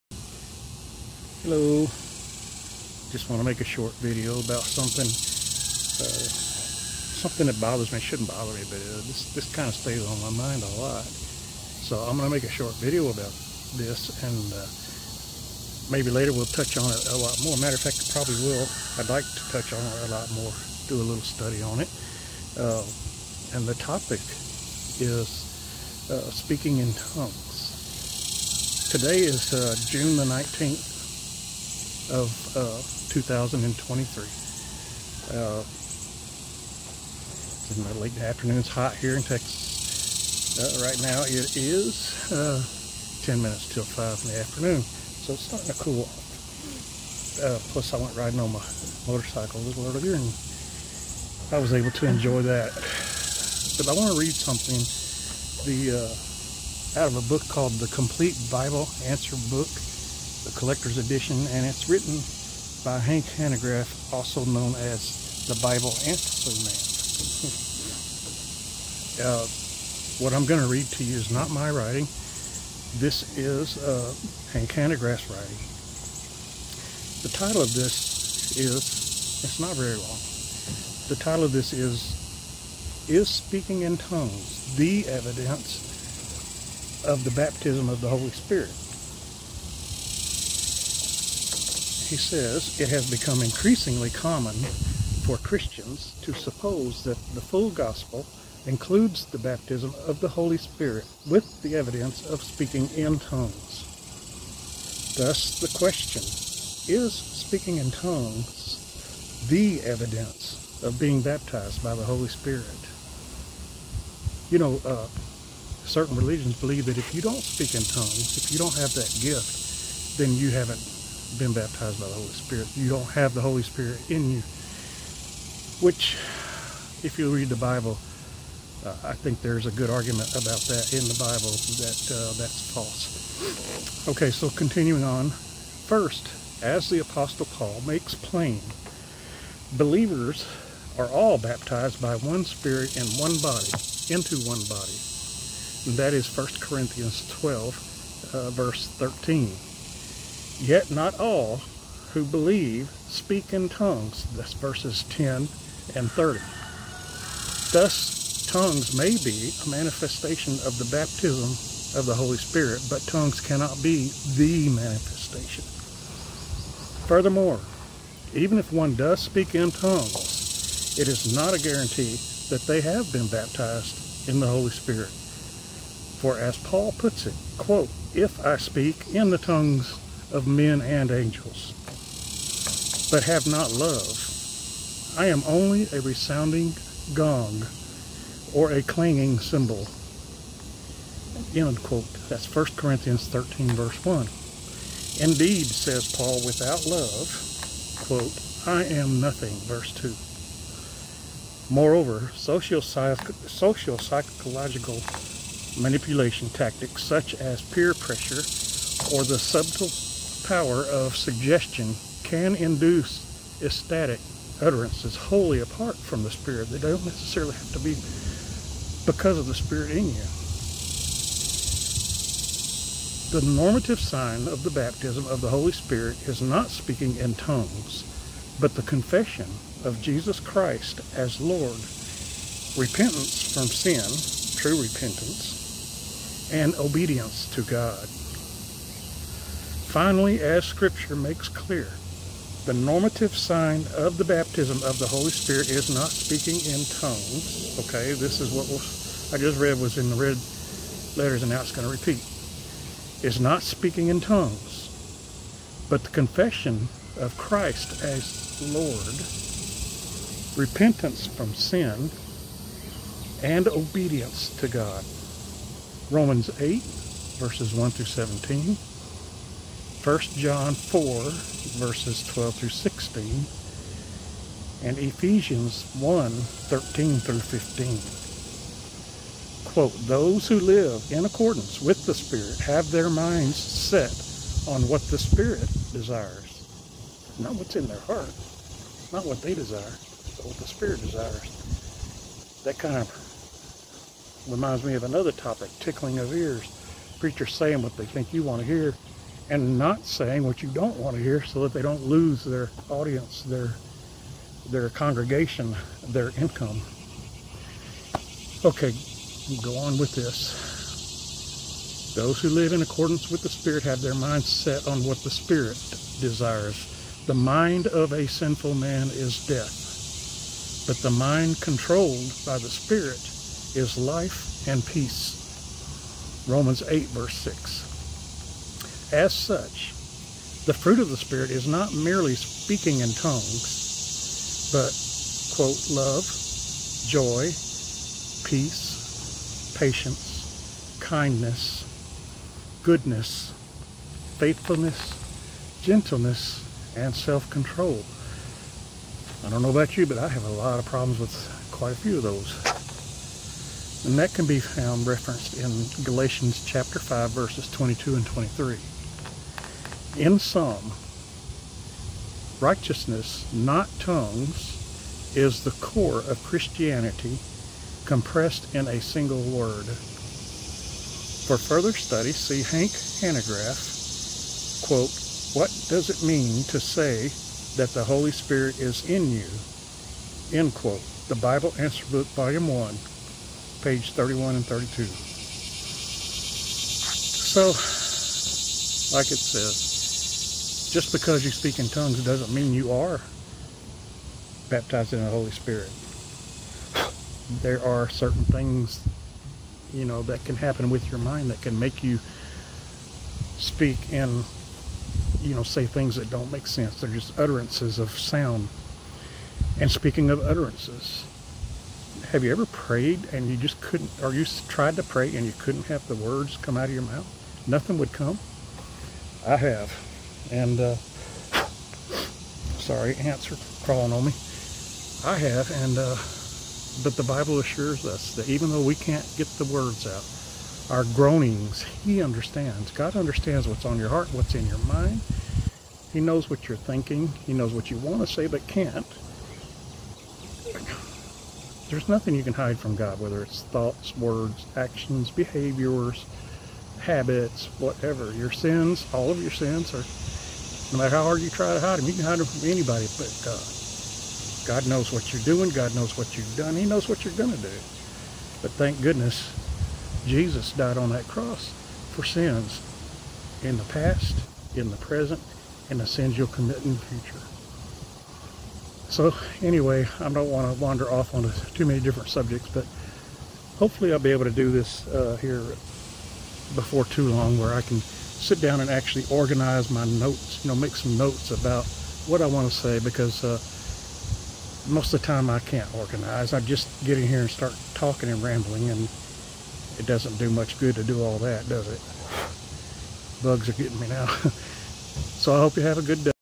June 19, 2023 - Speaking in tongues?
Is speaking in tongues the ONLY evidence of being filled with the Holy Spirit?